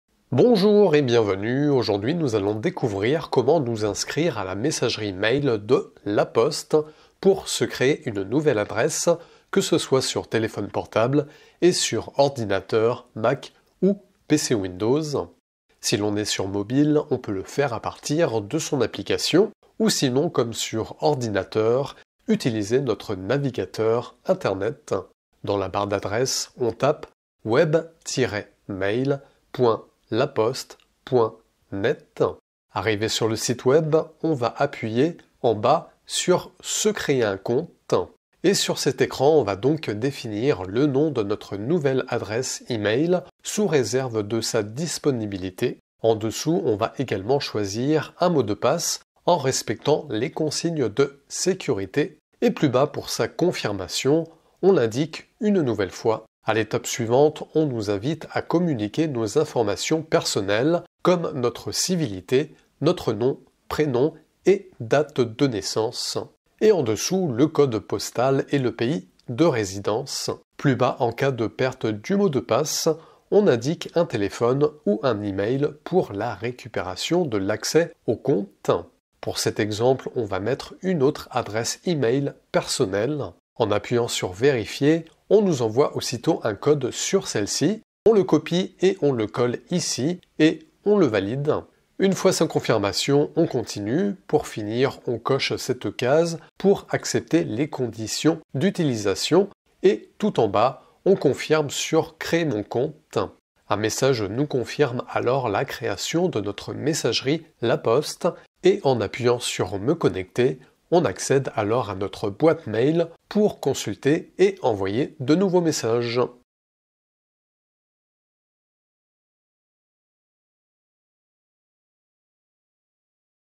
CREER messagerie LA POSTE, comment OUVRIR mon compte et ma BOITE MAIL LaPoste net pour une adresse
Créer messagerie La Poste, comment ouvrir mon compte et ma boite mail LaPoste.net pour une nouvelle adresse email, sur téléphone portable et ordinateur Mac ou PC.